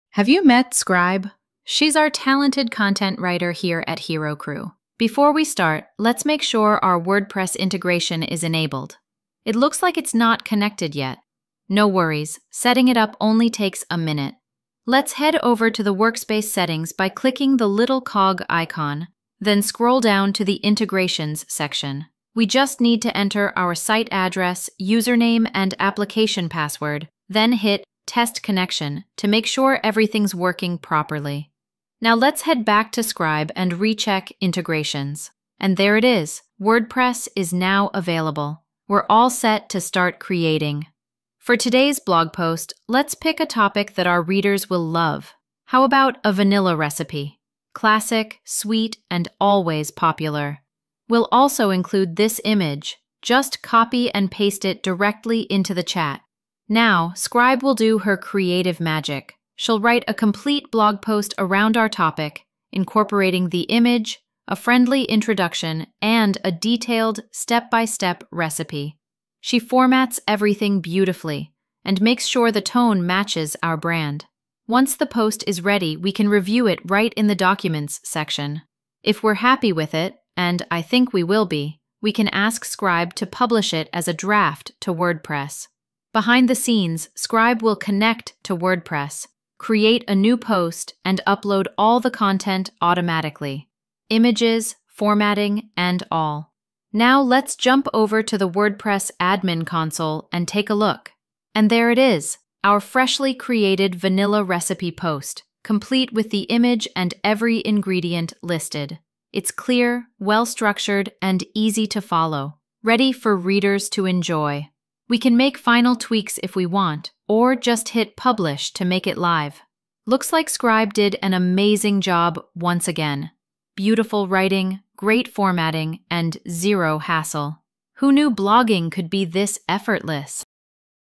Scribe - Wordpress integration
Have you met Scribe? She’s our talented content writer here at Hero Crew—and she’s always ready to help you create and publish blog posts straight to WordPress. Whether it’s a company update, a tutorial, or a recipe, Scribe makes the process simple and seamless.